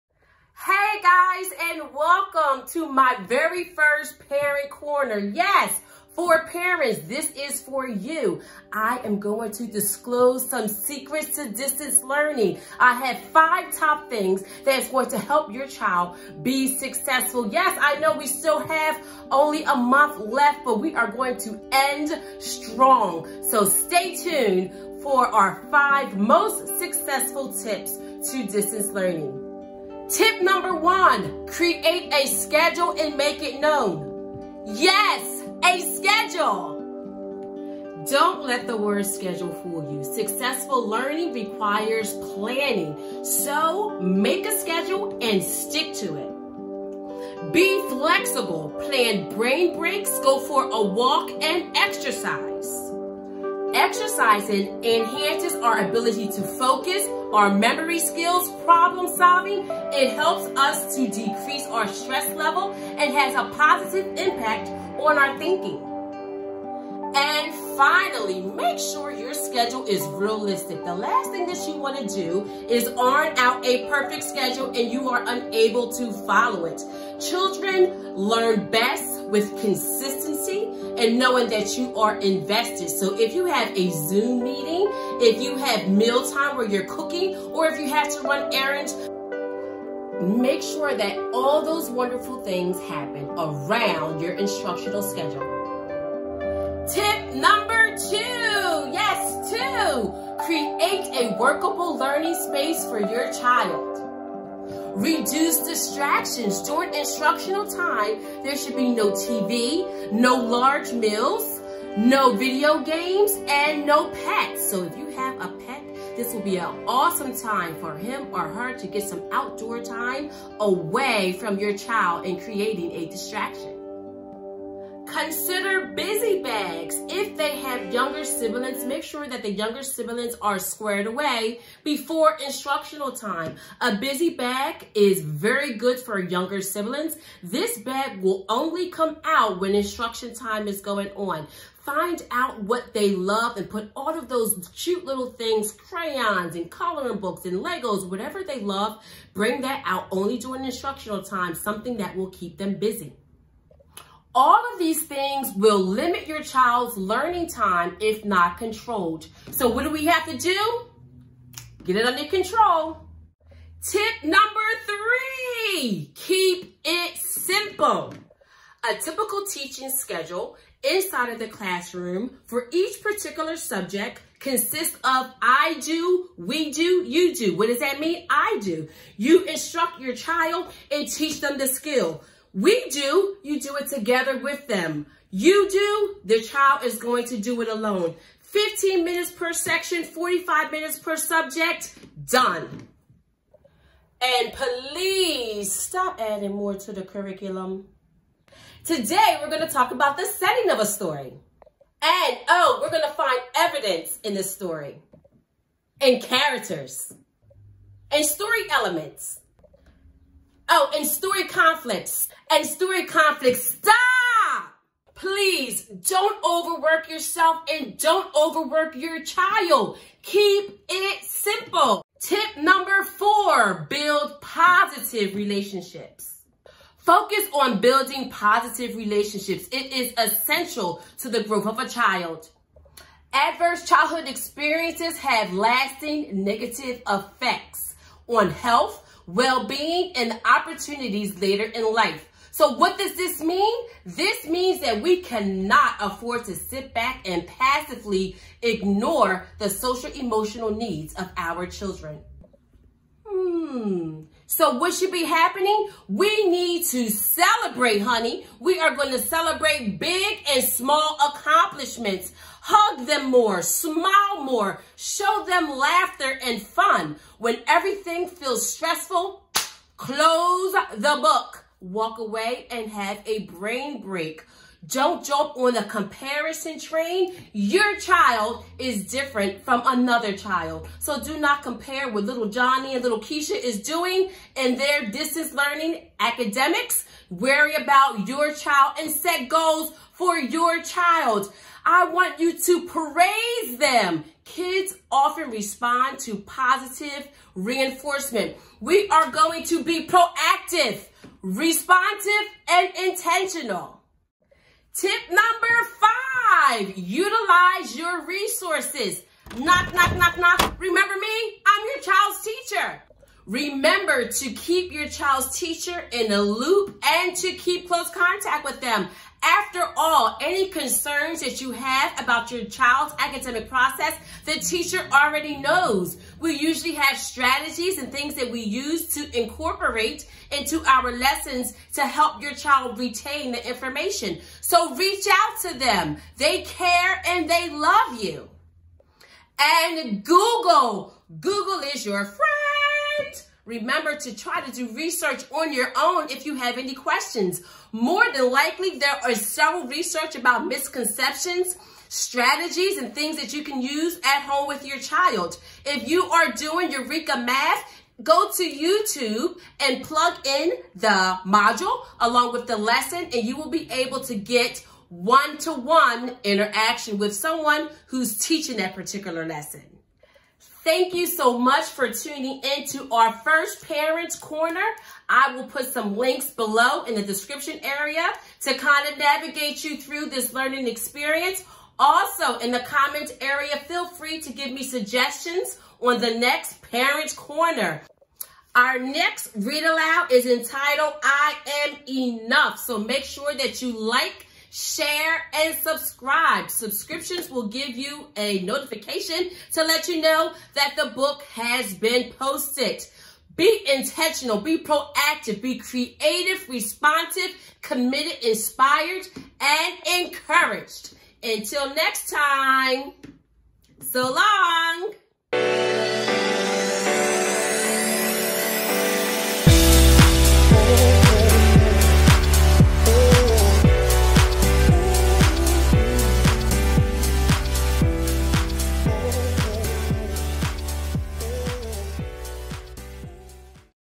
Secrets to Distance Learning
Resource for parents of children kinder-5th.
Welcome to the first parents corner from 'The Glam Girl Reads'

The rapid school closures that this pandemic has caused has taken many families by surprise. Suddenly, kids all over the country are being asked to learn from home, often through online learning, assignments sent via e-mail, or packets sent out by the school. This is a brand new experience for many of us. It’s not easy to convert homes into classrooms. But there are a few things you can do to get ready.

Thank you in advance for your support! WeCAN and WeWill